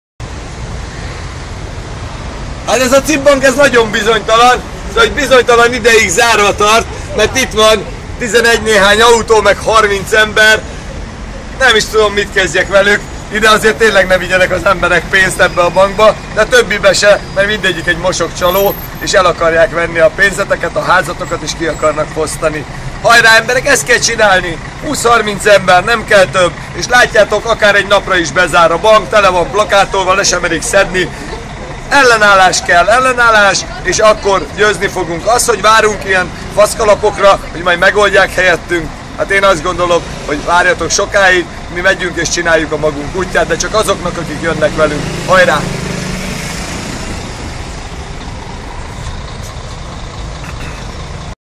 26/29 2013.10.20 Kásler és a sétakocsikázás
Néhány demonstráló sakkban tart egy CIB bankfiókot